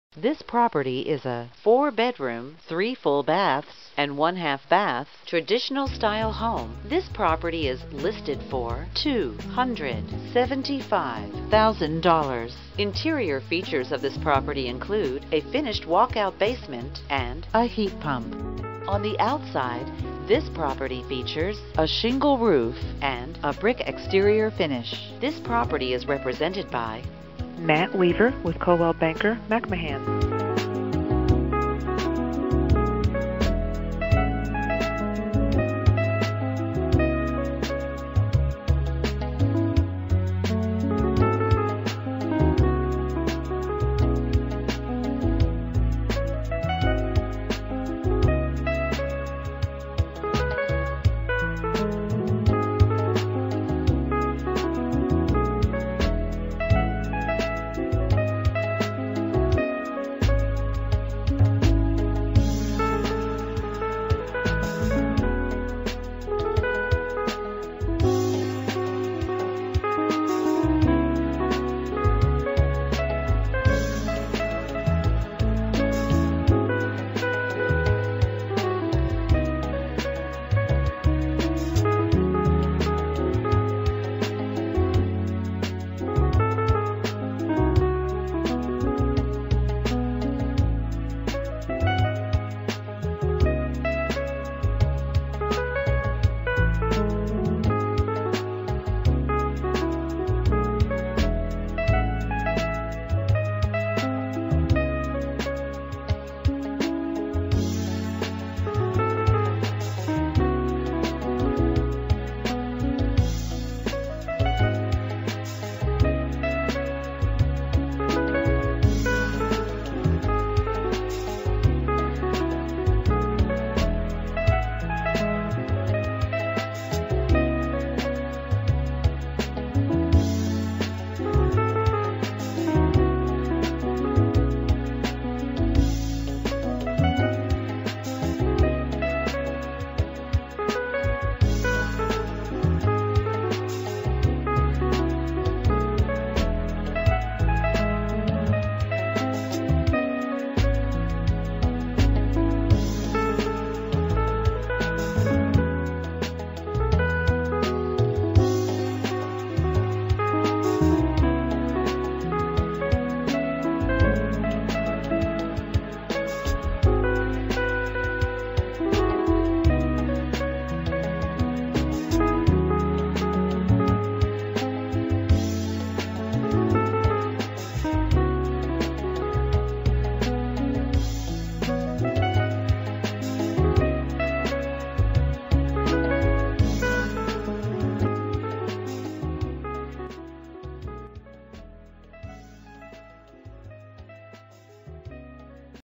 Real estate for sale in Lagrange Kentucky - MLS# 1387533
2606 Sycamore Run
Lagrange Kentucky 40031
MLS# 1387533

This all brick 2 story on 1 acre in Sycamore Run is move in ready!  Big rooms great flow.  Family Room with fireplace HUGE Eat-in Kitchen and Formal Dining Room have hardwood flooring.  1st floor laundry and half bath.  Master Suite with walk-in closet and private bath plus 3 large bedrooms and a 2nd full bath complete the second floor.  The walkout basement is finished with an office / guest room area a family room with great windows and a possible 5th BR (currently used as a workroom) with an attached 3rd full bath.  Outdoor living is one of the many great features of this home!  Full front porch a covered rear deck an open deck and a HUGE yard add to the charm and appeal of this great home.  2 car attached garage.  Plenty of storage.  Award winning Oldham County schools!